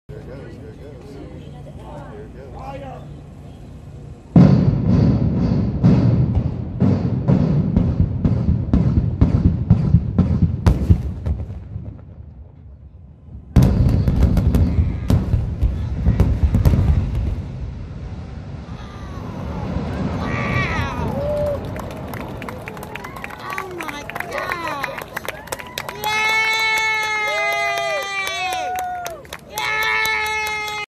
HISTORIC PASADENA BANK TOWER
The City of Pasadena has scheduled the implosion of the historic First Pasadena State Bank building for 7 a.m. Sunday after years of abandonment.